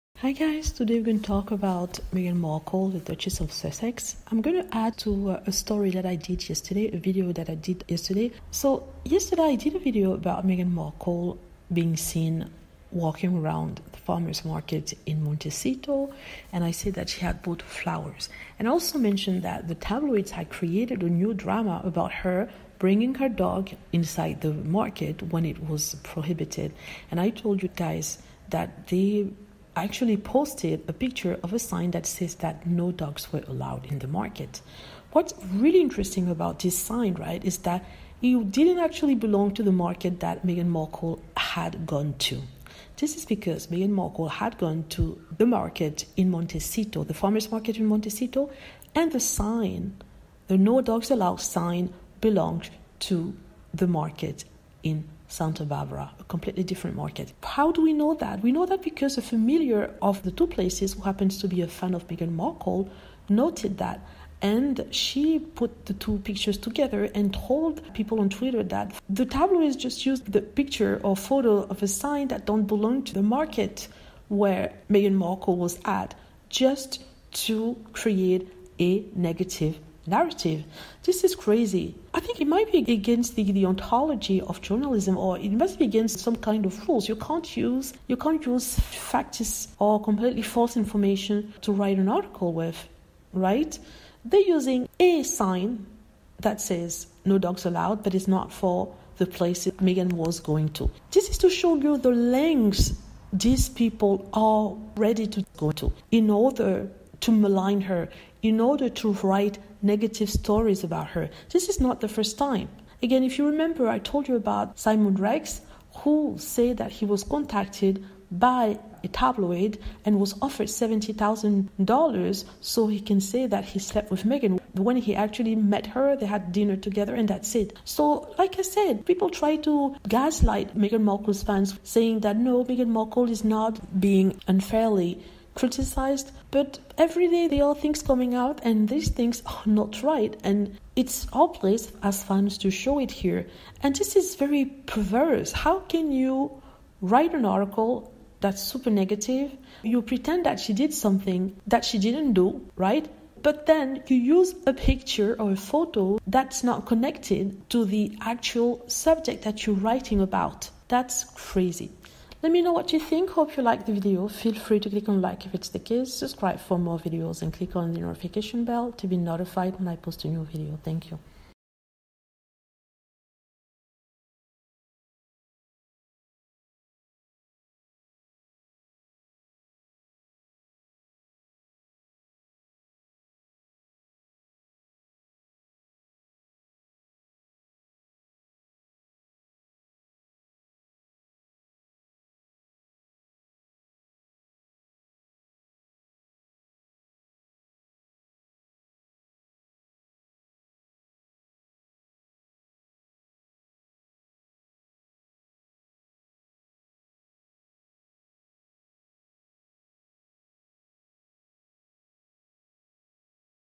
Meghan Markle and her dog in the tabloids again but you won't believe what they did this time.
meghan the duchess of sussex was accused of bringing her dog guy to the food market in montecito. the tabloids posted a no dogs alowed sign at the entrance of the famers market.